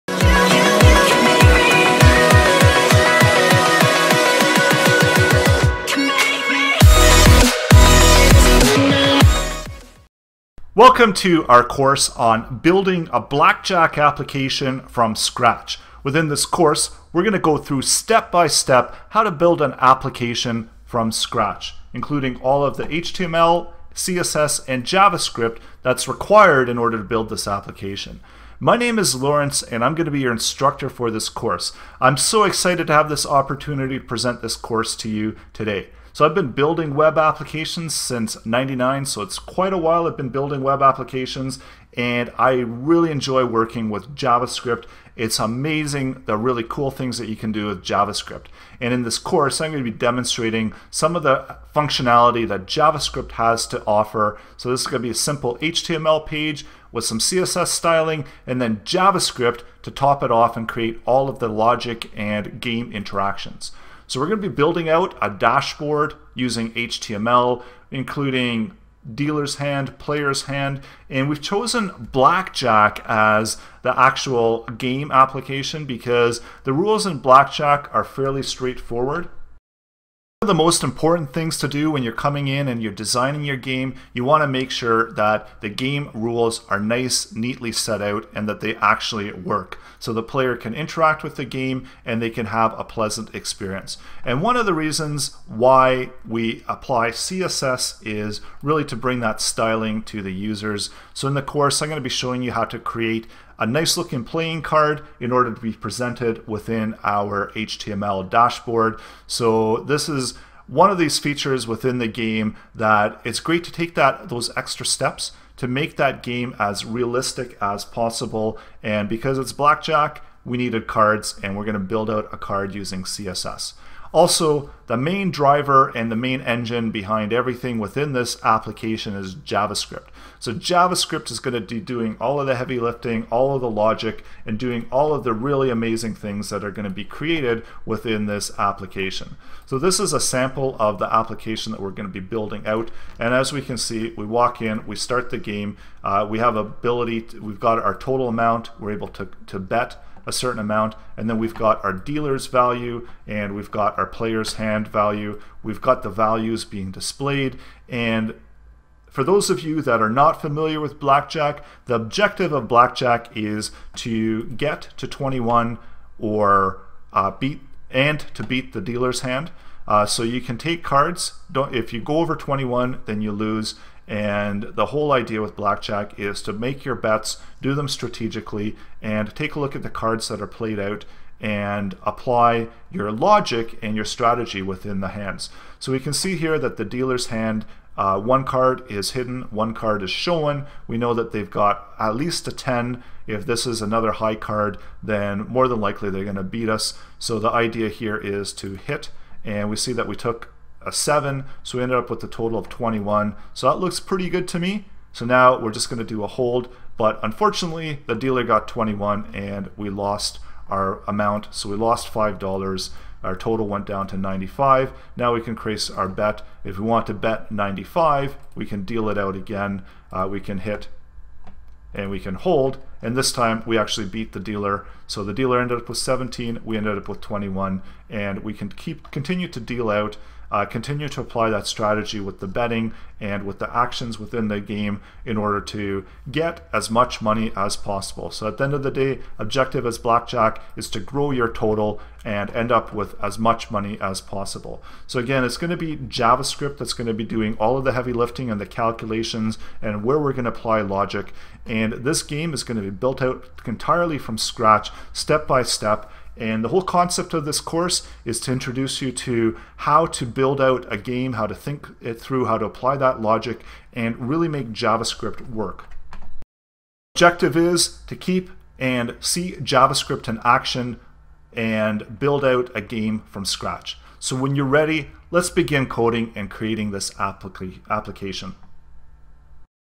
JavaScript HTML Game from Scratch Blackjack- Laurence Svekis|Learnfly
Explore the full course on Learnfly (special discount included in the link)

JavaScript HTML Game from Scratch Blackjack

Learn how to create a web application from scratch.  Apply JavaScript to HTML and CSS to build a Blackjack application

Learn how to build a playable Blackjack web based application from scratch!

Using HTML, CSS and JavaScript this course will show you how to create a game from scratch.  One of the best ways to learn how to use JavaScript is to apply it to projects.  This course show you the steps and process from a blank file to building out the entire game.

This course is designed for beginners with some basic JavaScript, HTML and CSS experience required.  We will be reviewing only the JavaScript functionality required to build the game and demonstrating how to apply the code. This course does not cover HTML, CSS and JavaScript in their entirety but covers the use of them in relation to building a game.

Start with the basics of what is needed, apply logic and build out a working web application.  Test your application, tweak fine tune and get it working as designed.

Start off building a deck of cards in JavaScript

Learn how to generate values of cards to use within your application.

Prepare to build the game.  Define the game rules and what you need in order to get that functionality.

Learn how to build a playing card visually with CSS

Create the game play using JavaScript applying logic

Add player actions and decision points

Create the logic for the dealer

Put it all together to play Blackjack

Source Code is included, step by step instruction to demonstration how to build an application.

Taught by an instructor with over 18 years of web development experience.

I'm here to help you learn how to write JavaScript and ready to answer any questions you may have.

This course showcases how to build a fun interactive application.  When you are ready

Follow Learnfly Academy